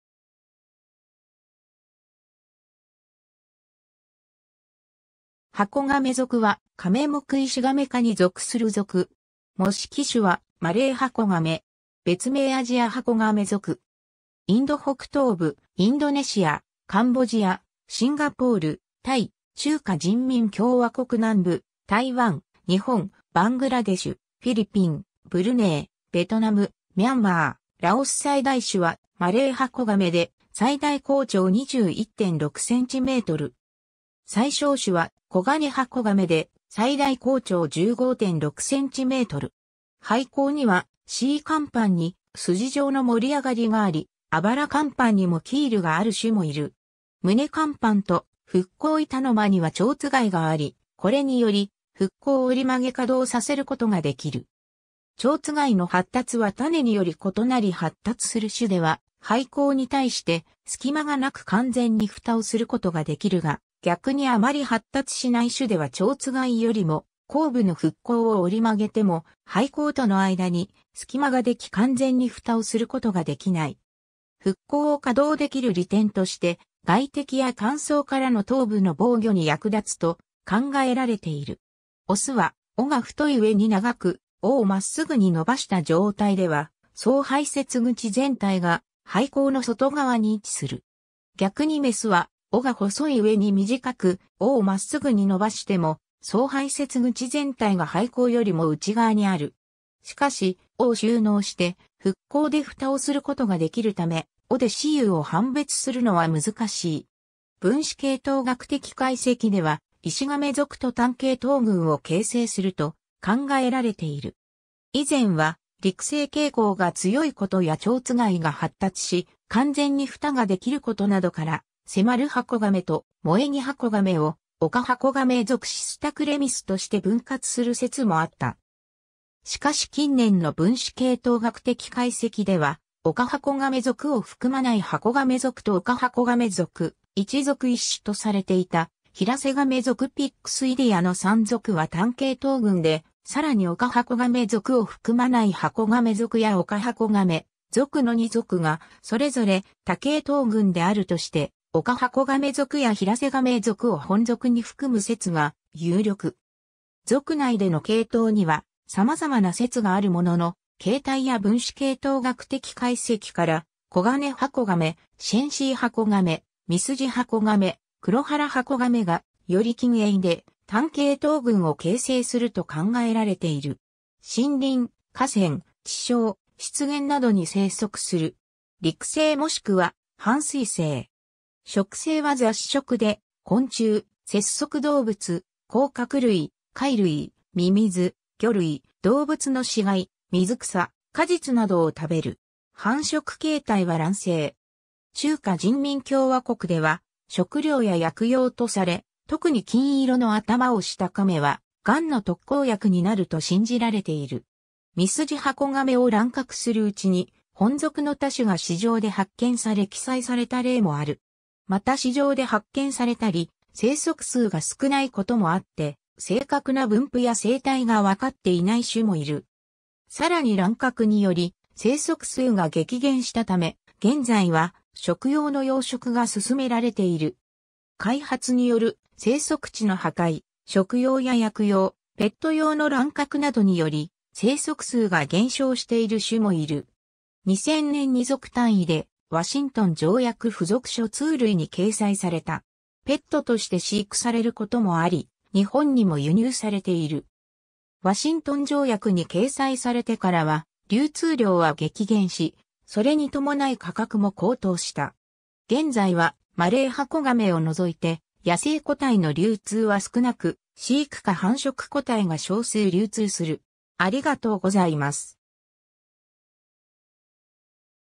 ハコガメ属
ハコガメ属（ハコガメぞく、Cuora）は、カメ目イシガメ科に属する属。
模式種はマレーハコガメ。
別名アジアハコガメ属。
 インド北東部、インドネシア、カンボジア、シンガポール、タイ、中華人民共和国南部、台湾、日本（石垣島、西表島）、バングラデシュ、フィリピン、ブルネイ、ベトナム、ミャンマー、ラオス 最大種はマレーハコガメで最大甲長21.6cm。
最小種はコガネハコガメで最大甲長15.6cm。
（以上は2000年までに採集例が6例しかなく、絶滅したとされその後に再発見されたユンナンハコガメを除く。）背甲には椎甲板に筋状の盛り上がり（キール）があり、肋甲板にもキールがある種もいる。
胸甲板と腹甲板の間には蝶番があり、これにより腹甲を折り曲げ可動させることができる。
蝶番の発達は種により異なり発達する種では背甲に対して隙間が無く完全に蓋をすることができるが、逆にあまり発達しない種では蝶番よりも後部の腹甲（後葉）を折り曲げても背甲との間に隙間ができ完全に蓋をすることができない。
腹甲を可動できる利点として外敵や乾燥からの頭部の防御に役立つと考えられている。
 オスは尾が太いうえに長く、尾をまっすぐに伸ばした状態では総排泄口全体が背甲の外側に位置する。
逆にメスは尾が細いうえに短く、尾をまっすぐに伸ばしても総排泄口全体が背甲よりも内側にある。
しかし尾を収納して腹甲で蓋をすることができるため、尾で雌雄を判別するのは難しい。
 分子系統学的解析では、イシガメ属（クサガメ属やハナガメ属も含めた）と単系統群を形成すると考えられている。
 以前は陸棲傾向が強いことや蝶番が発達し完全に蓋ができることなどから、セマルハコガメとモエギハコガメをオカハコガメ属Cistoclemmysとして分割する説もあった。
しかし近年の分子系統学的解析ではオカハコガメ属を含まないハコガメ属とオカハコガメ属、1属1種とされていたヒラセガメ属Pyxideaの3属は単系統群で、さらにオカハコガメ属を含まないハコガメ属やオカハコガメ属の2属がそれぞれ多系統群であるとして、オカハコガメ属やヒラセガメ属を本属に含む説が有力。
 属内での系統には様々な説があるものの、形態や分子系統学的解析からコガネハコガメ、シェンシーハコガメ、ミスジハコガメ、クロハラハコガメがより近縁で単系統群を形成すると考えられている。
 森林、河川、池沼、湿原などに生息する。
陸棲もしくは半水棲。
 食性は雑食で、昆虫、節足動物、甲殻類、貝類、ミミズ、魚類、動物の死骸、水草、果実などを食べる。
 中華人民共和国では食料や薬用とされ、特に金色の頭をしたカメ（ミスジハコガメ）は癌の特効薬になると信じられている。
ミスジハコガメを乱獲するうちに、本属の他種が市場で発見され記載された例もある。
また市場で発見されたり生息数が少ないこともあって、正確な分布や生態がわかっていない種もいる。
さらに乱獲により生息数が激減したため、現在は食用の養殖が進められている。
 開発による生息地の破壊、食用や薬用、ペット用の乱獲などにより生息数が減少している種もいる。
2000年に属単位でワシントン条約付属書II類に掲載された。
ワシントン条約に掲載されてからは流通量は激減し、それにともない価格も高騰した。
現在はマレーハコガメを除いて野生個体の流通は少なく、飼育下繁殖個体が少数流通する。